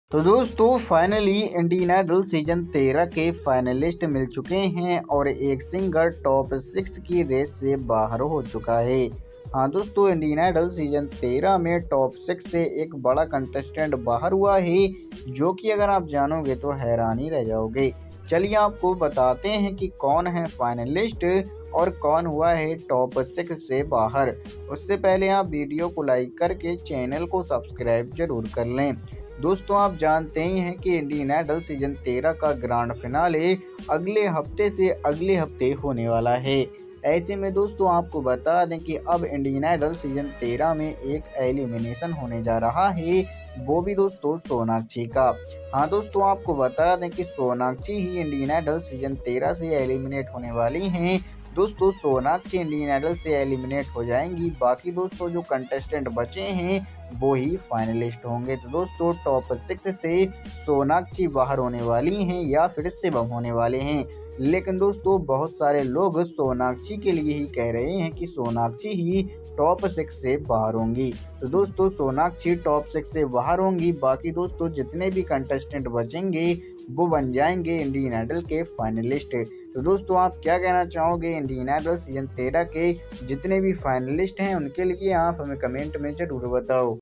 ये हुआ इंडियन आइडल की टाॅप 6 की रेस से बाहर | Indian Idol 13 Finalist Name Revealed | Rishi Chirag
हुआ इंडियन आइडल की टाॅप 6 की रेस से बाहर | Indian Idol 13 Finalist Name Revealed | Rishi Chirag